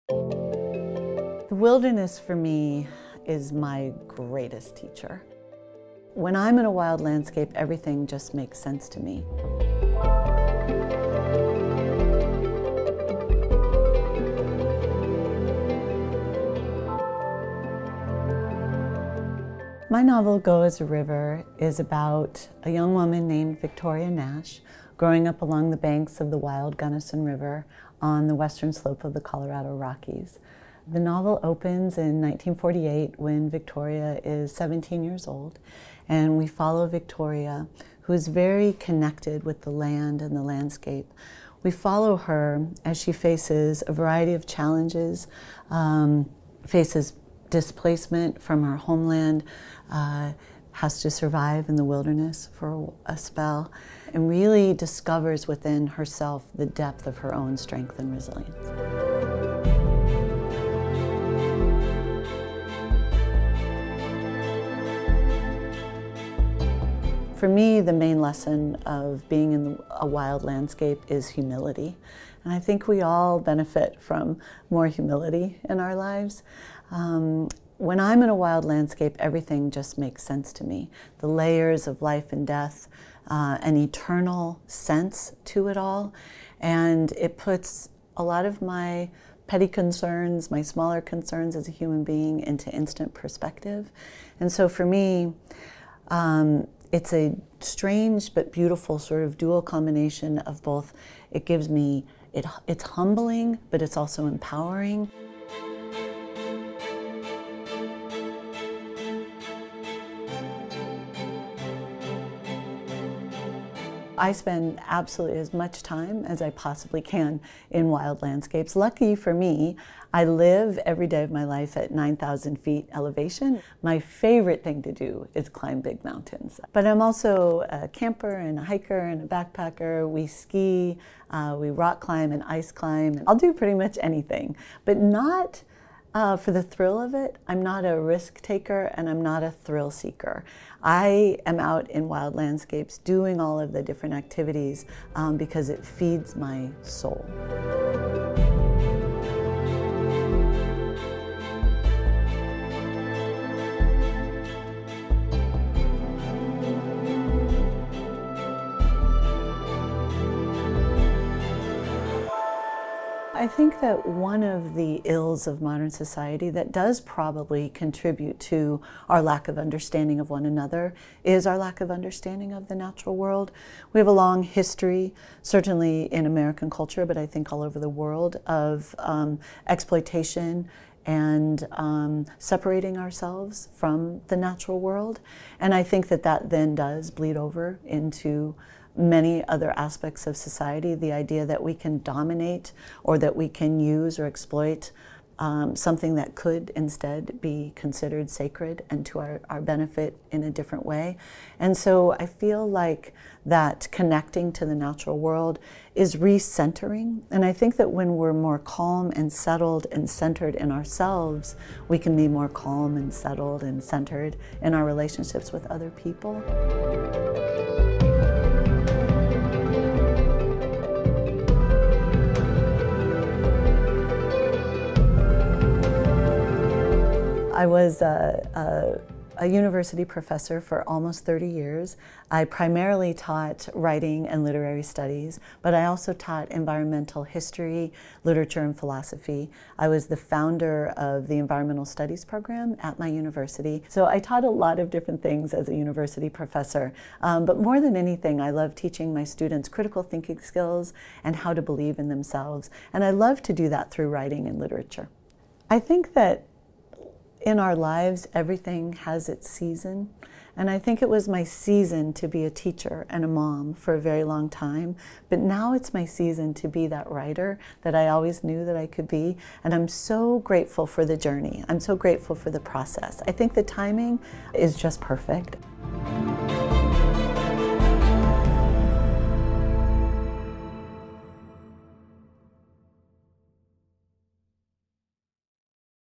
Shelley Read - "So weit der Fluss uns trägt" (Buchtrailer) | C. Bertelsmann
»Erschütternd, Mut machend und absolut unvergesslich. Ein Roman wie eine Naturgewalt.« Bonnie Garmus

Am Fuße der Berge Colorados strömt der Gunnison River an einer alten Pfirsichfarm vorbei. Hier lebt in den 1940ern die 17-jährige Victoria mit ihrem Vater und ihrem Bruder in rauer Abgeschiedenheit. Doch der Tag, an dem sie dem freiheitsliebenden Wil begegnet, verändert alles. Bald ist Victoria gezwungen, das Leben, das sie kennt, aufzugeben und in die Wildnis zu fliehen. Dort muss sie ums Überleben kämpfen – um ihr eigenes und um das ihres ungeborenen Kindes. Als sie endlich die Kraft findet, neu anzufangen, droht der Fluss, alles zu zerstören, was ihrer Familie seit Generationen ein Zuhause war.

Ein lebenskluger Roman über unsere Verbindung zur Natur, über Familie und die Stärke einer Frau, die Unglaubliches erlebt und doch niemals den Mut verliert.